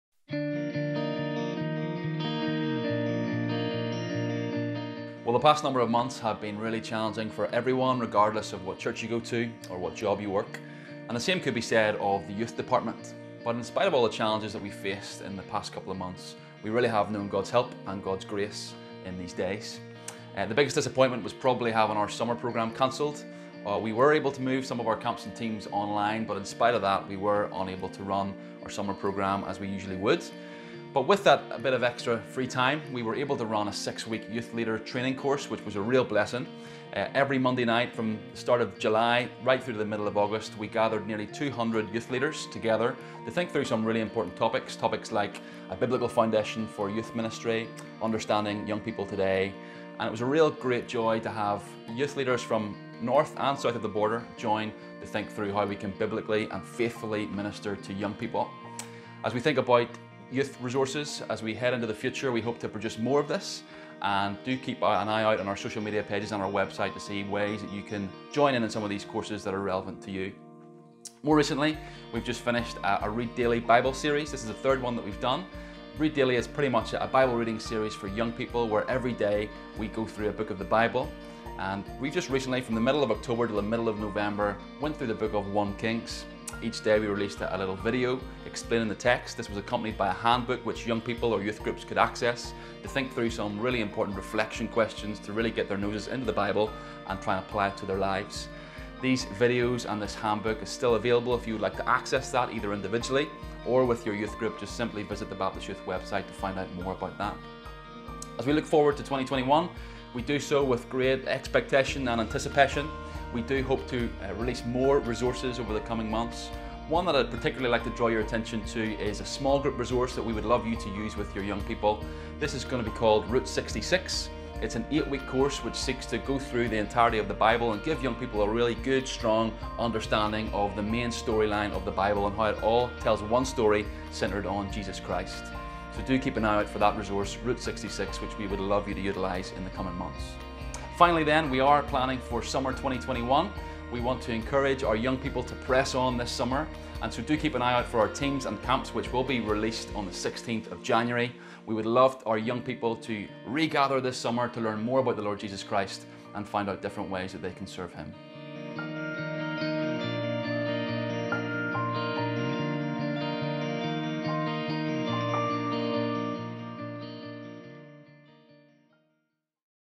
Baptist Youth 2020 - Promo
A short message from Matt Campbell, Baptist Youth Director.